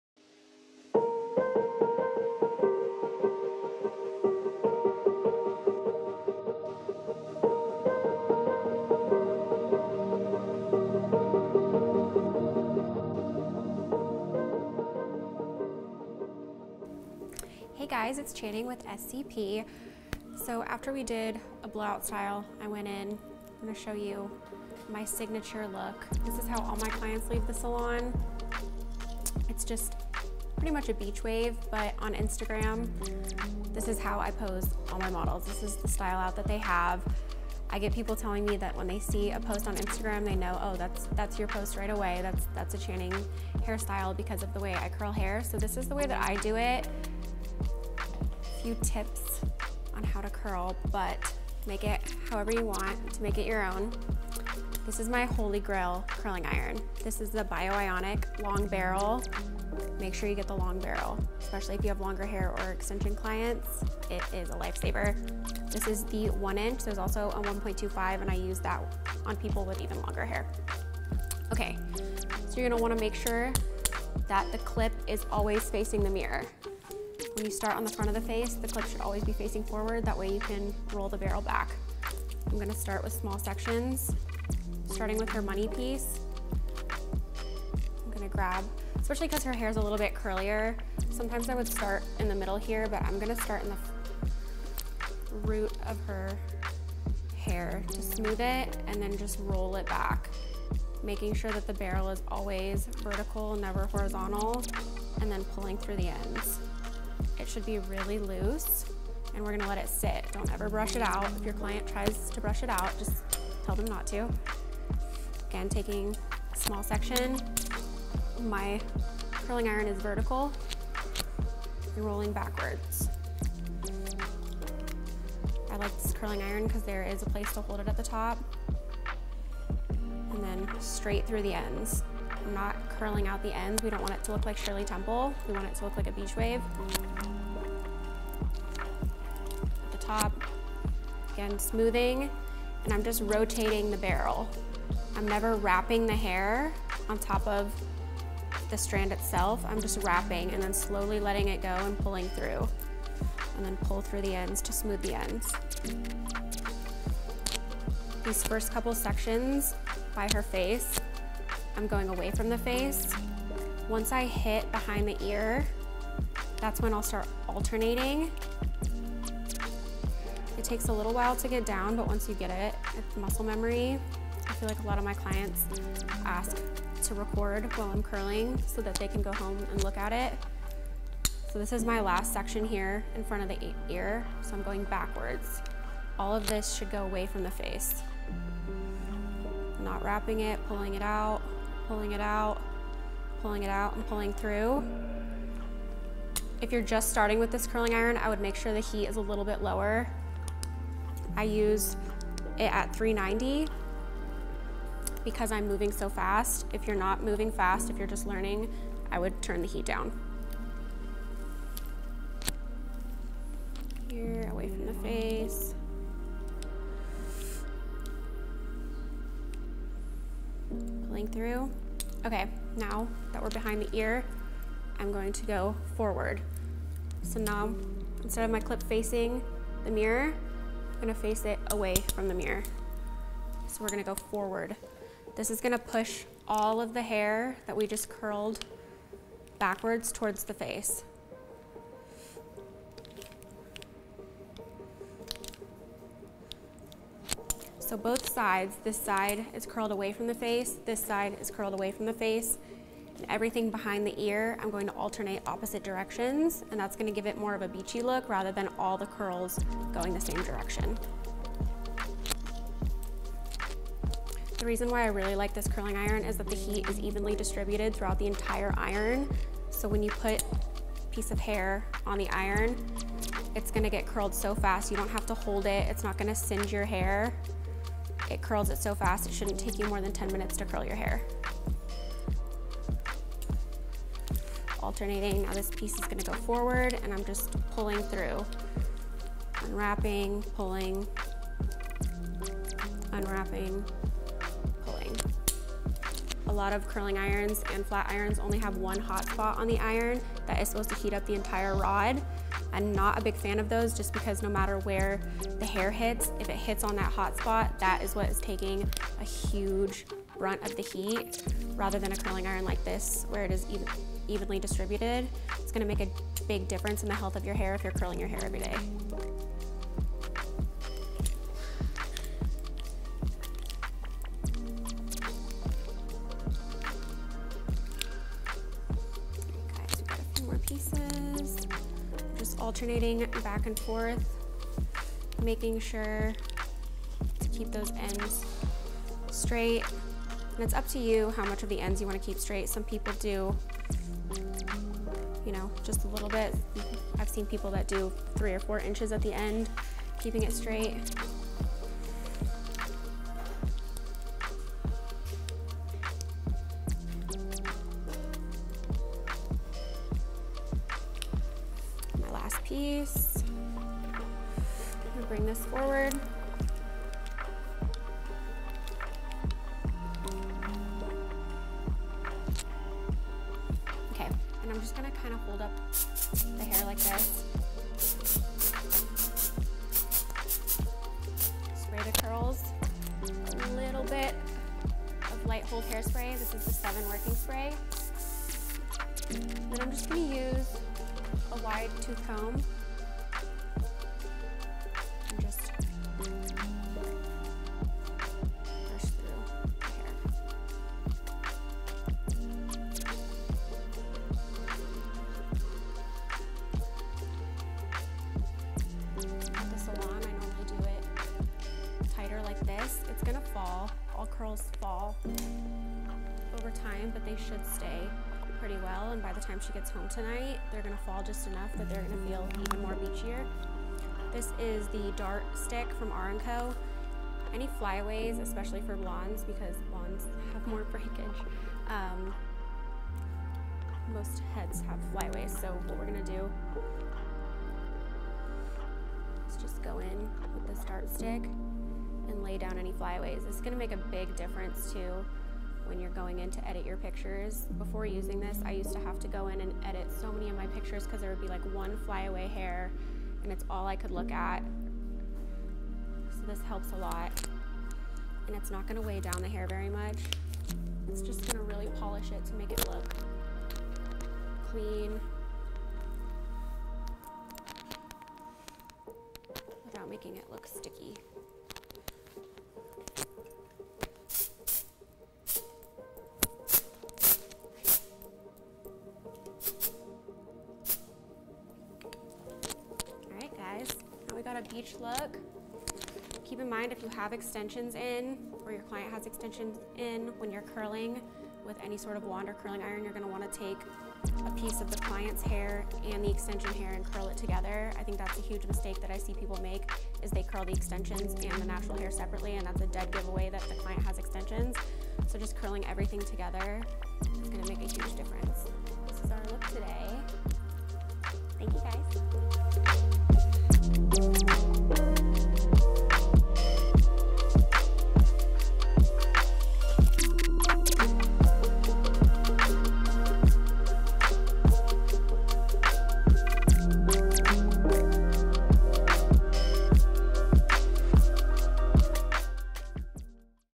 How to: Curl for BEACHY WAVY HAIR | Full Styling Video | Dope Hair Education
Learn how to create a Beach Wave look with Channing. This is a step-by-step hair tutorial by our educator Channing.

DOPE Hair Education in collaboration with SCP
Hair Educator: Channing Brown
Filmmaker: Carlos Ku King
Marketing Agency: DARO Studio